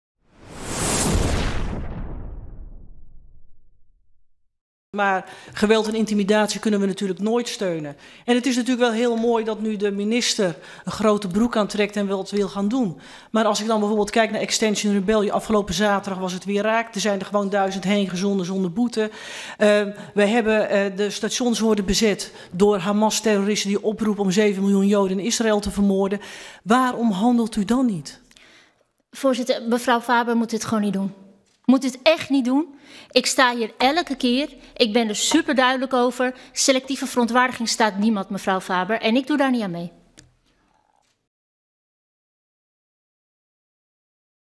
PVV'er Faber maakt Yesilgöz WOEST! 'Klimaat-gekkies komen overal mee weg!'
PVV'er Faber maakt Yesilgöz W0EST! 'Klimaat-gekkies komen overal mee weg!'